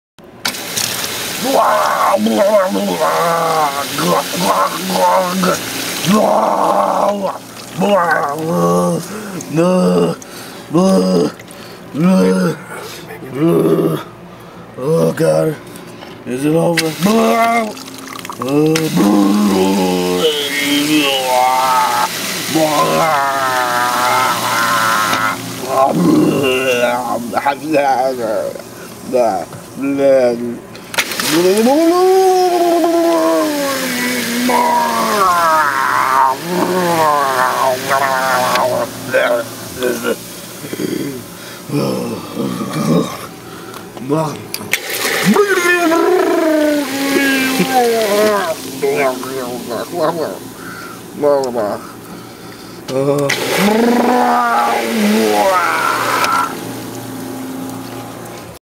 Barf Cup
This is a genius invention, a range of alcohol containers that allow you to drink as much intoxicating booze as possible and never get drunk. What's more, it even barfs up at the end of the evening so you don't feel left out. WANT.

(Uploaded this to Punchbaby and my YT channel before 'The Splendiferous Barfing Cup' - broke on Reddit and made a great video even greater. So credit to original author eriktheoz - Love this gem :)